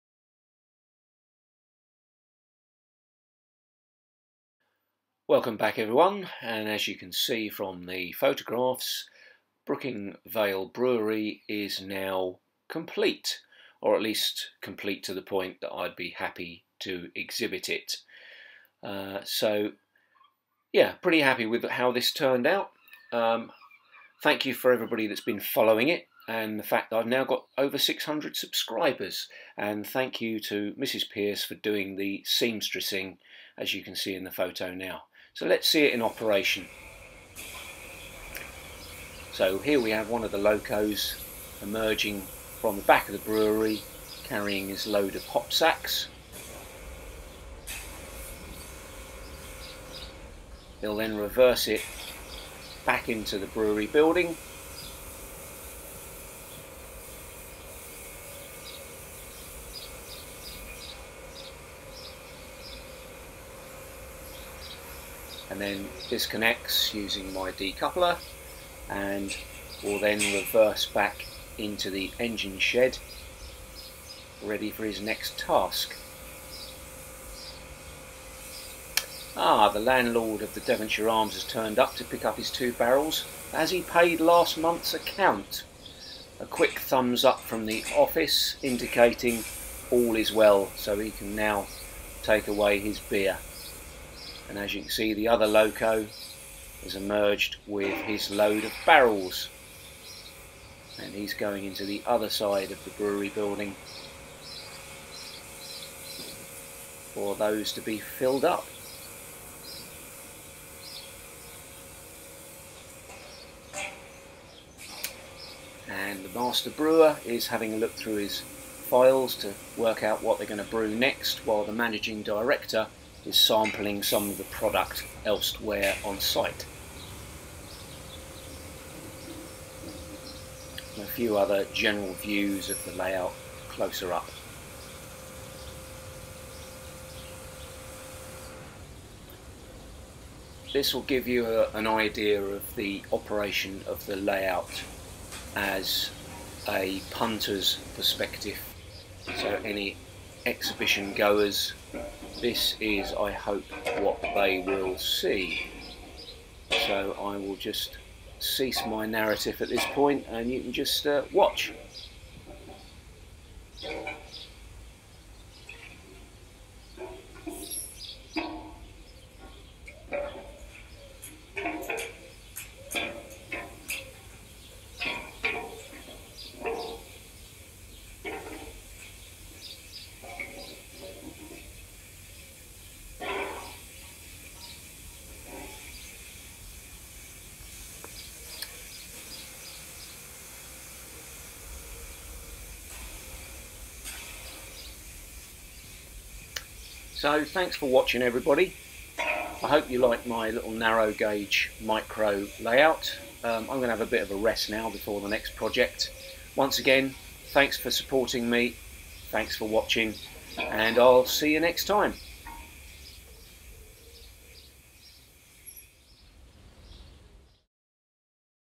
O 16 5 micro layout, The Brooking Vale Brewery is now complete!
My 900x400mm micro narrow gauge layout is now finished and ready for exhibition!